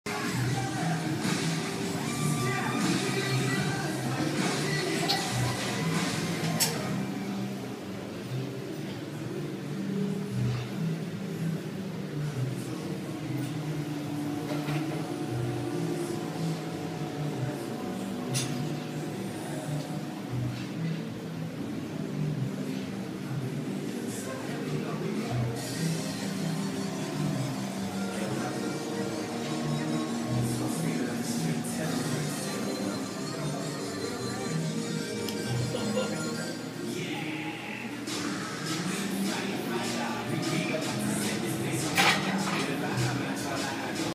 5x10@135 squats
First time squatting in 6 weeks since impinging my right knee. Going slow and as deep as possible.